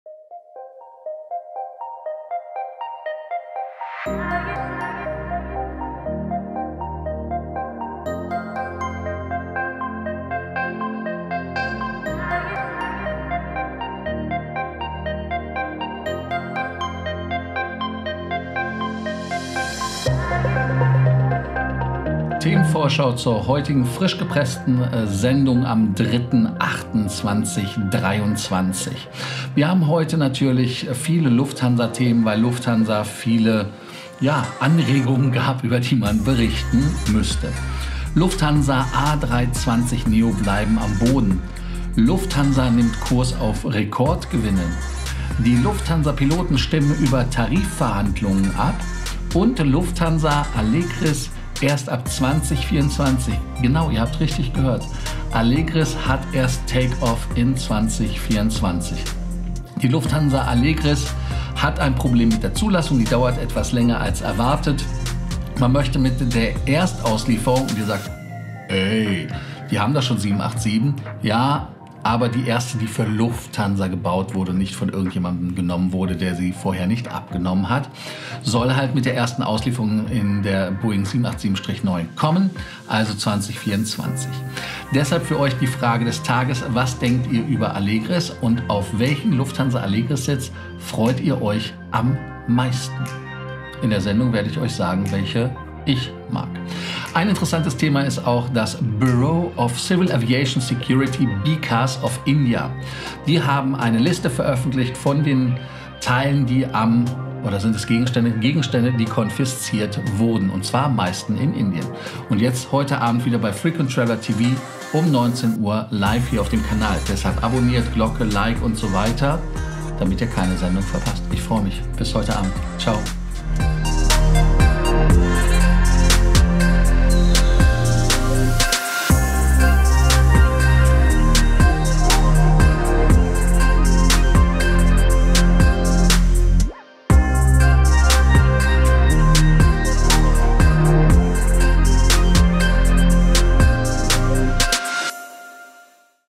Themenvorschau: ✈️ Lufthansa Allegris Take Off in 2024!
Die Themenvorschau auf die heutige Sendung um 19 Uhr deutscher Zeit.

Clubhouse - @LFCorsten